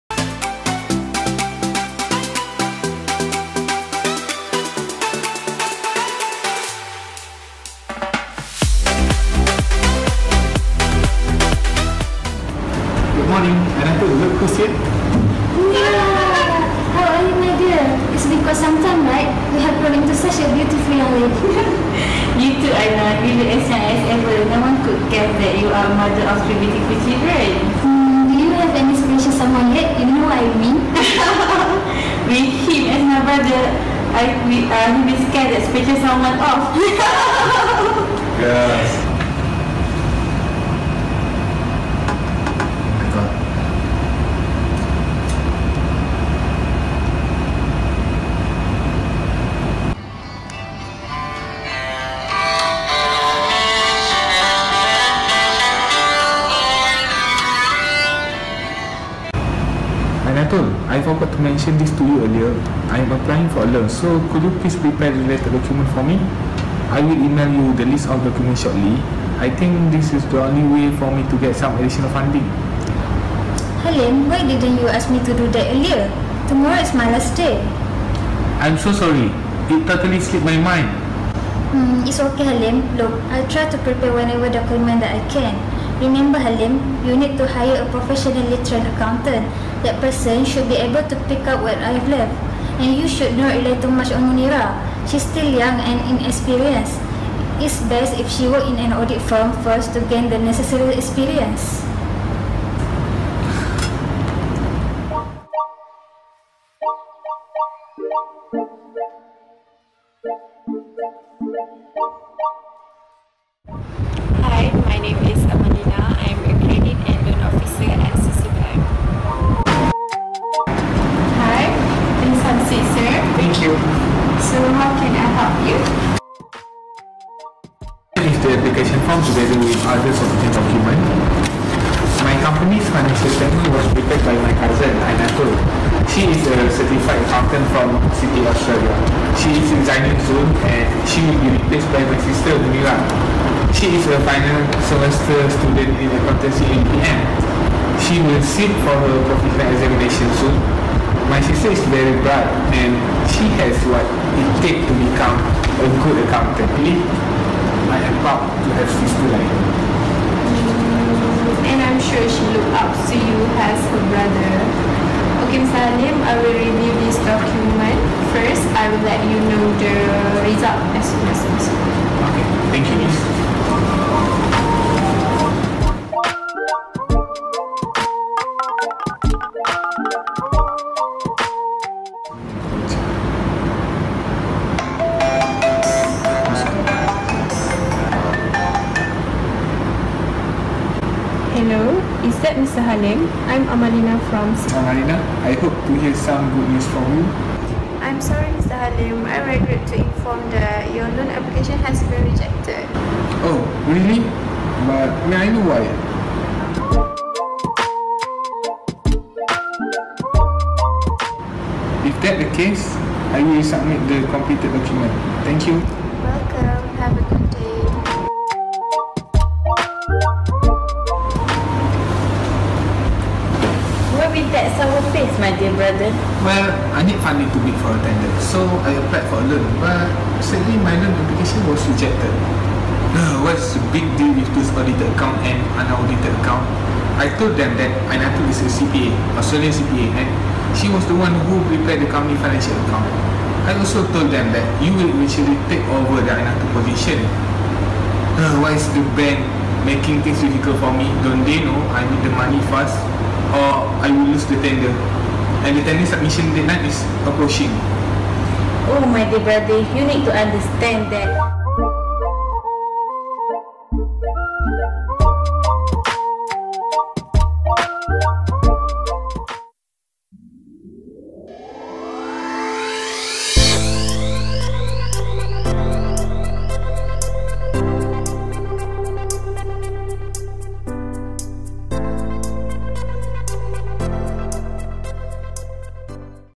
PutraMOOC || Why Audit? (Part II).
A case that highlights the importance of auditing in the absence of statutory requirements, continues.